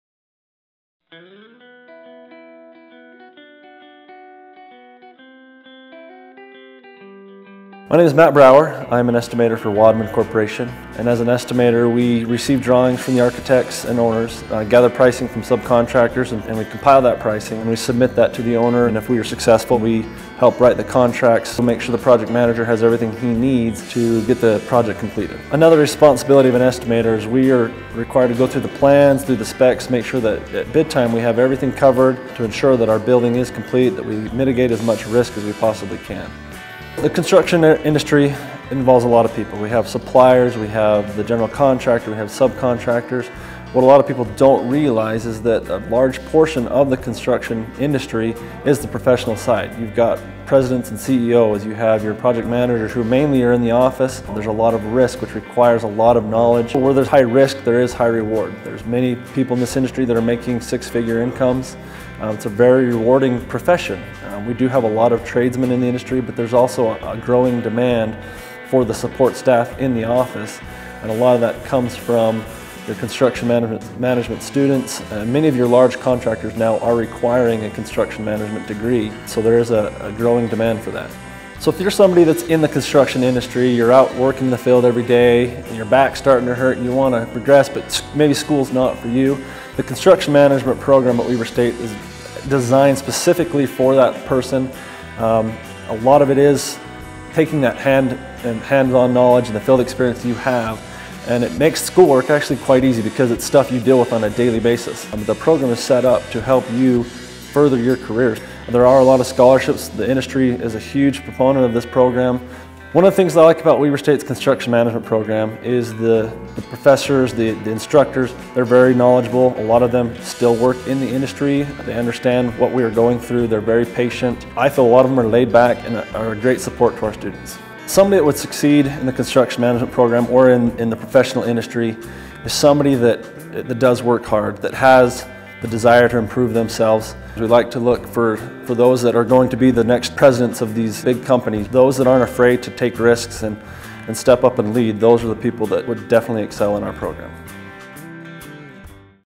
Construction Management Degree: Jobs in Construction
Construction Management Technology: Matt Brower works as an Estimator for Wadman Corp. By getting his Construction Management degree at Weber State University we was able to get a degree that he uses everyday.